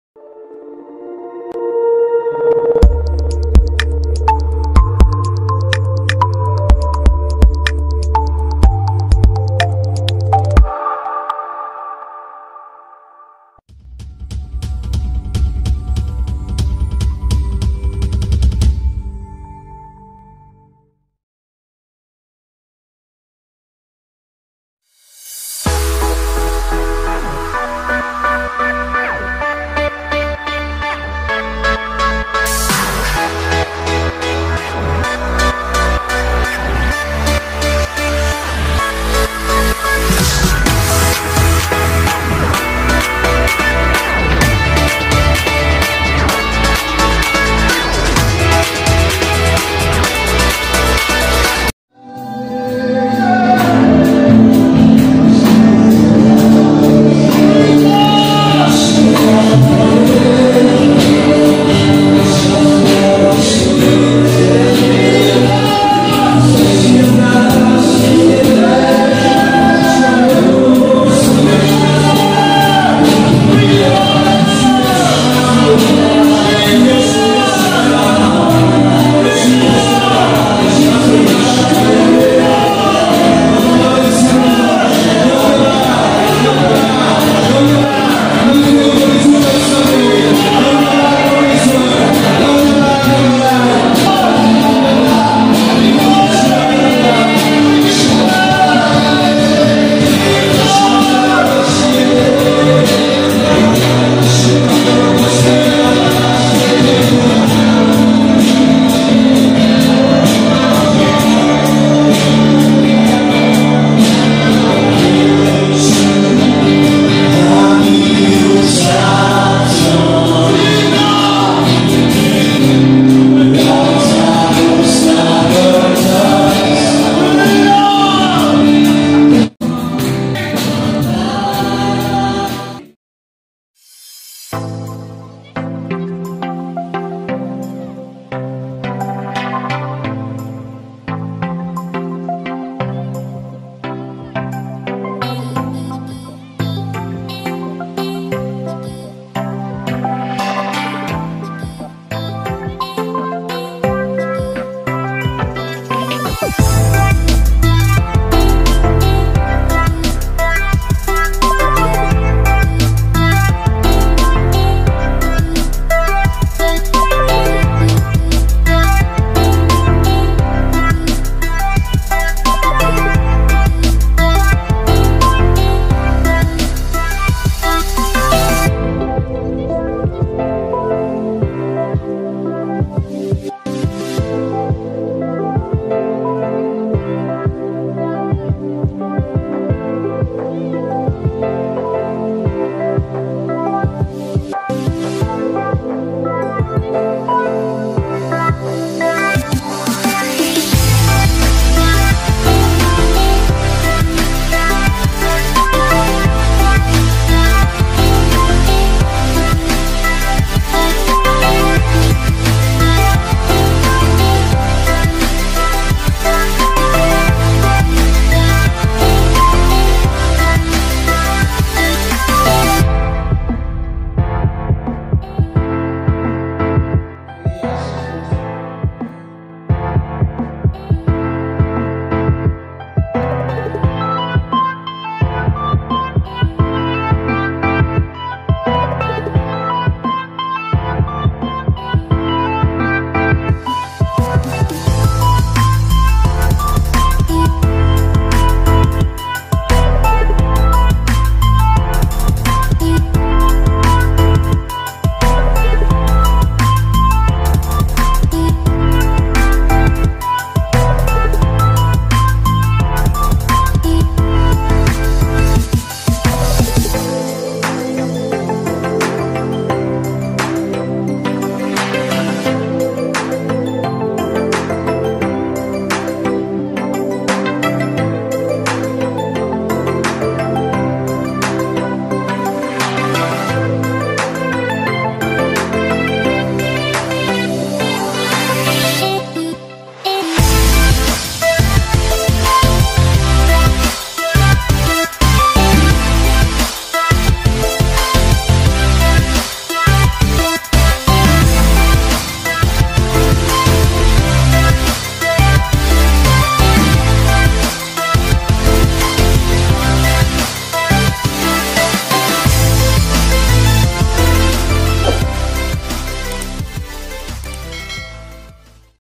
After Lockdown Church Re-open // Bethlehem Fellowship church // Vlog
Feeling blessed Today after long gap Church Re- open

For where two or three are gathered in my name, there am I among them.”

"Then had the churches rest throughout all Judaea and Galilee and Samaria, and were edified; and walking in the fear of the Lord, and in the comfort of the Holy Ghost, were multiplied."